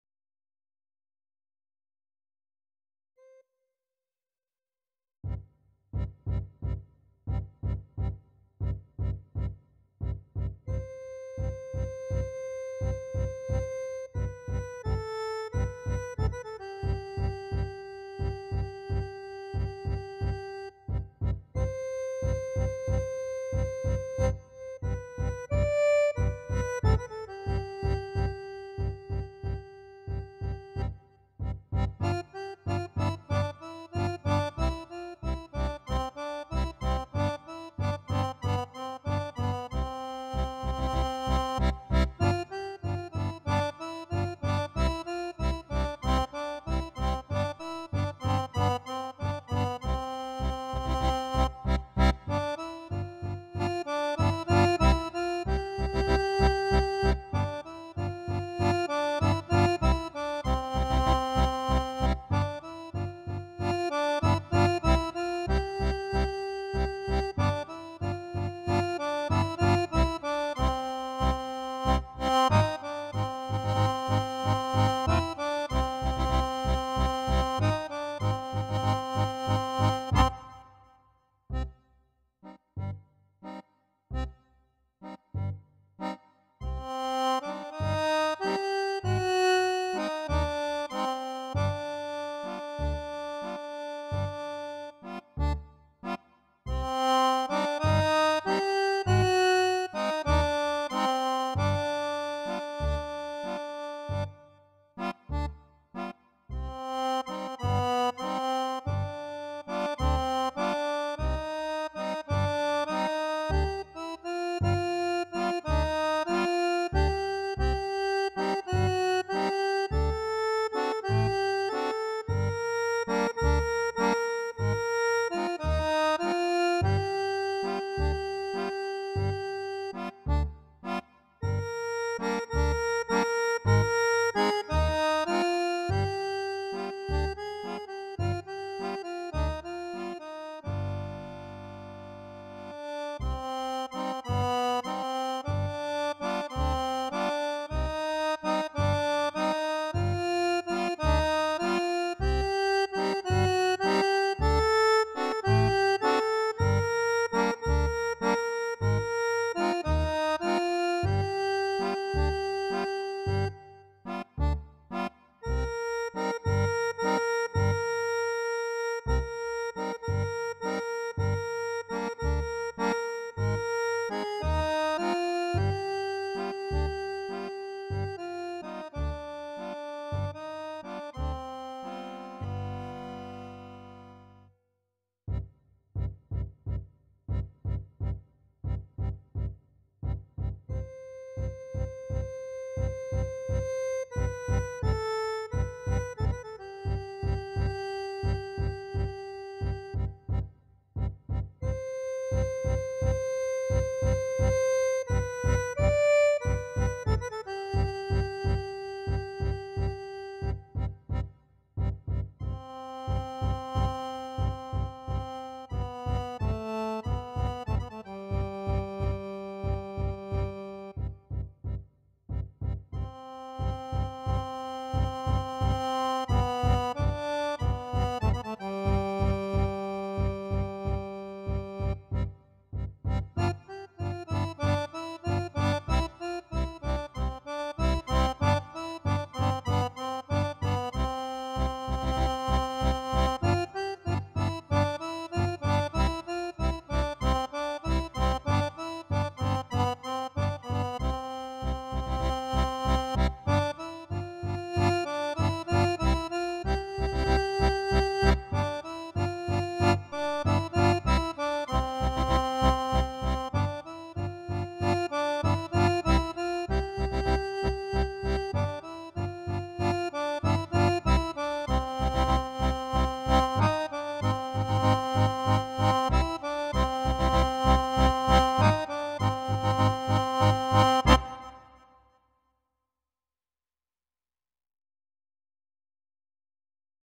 tango bolero kubo
tango bolero kubo apprise d'apres la video de kubo avec une aquarelle de nos falaises normande.